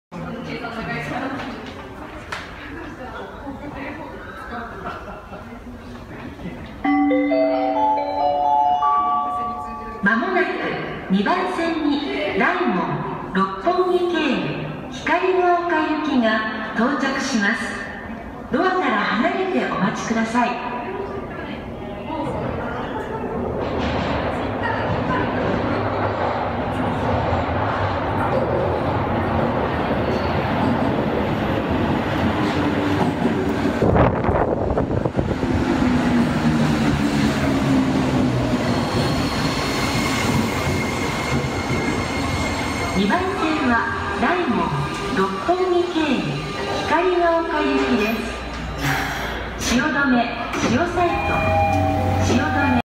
都営地下鉄大江戸線 光が丘ゆき 汐留駅 到着
도영 지하철 오에도선 히카리가오카행 시오도메역 도착
Toei Ōedo Line bound for Hikarigaoka arriving at Shiodome Station

撮影日付: 2016年 3月 21日
車両番号: 28(東京都交通局12-000形電車)

촬영일자: 2016년 3월 21일
편성: 28(도쿄도 교통국 12-000형 전동차)

Date of recording: March 21, 2016
Car number: 28(Toei 12-000 series)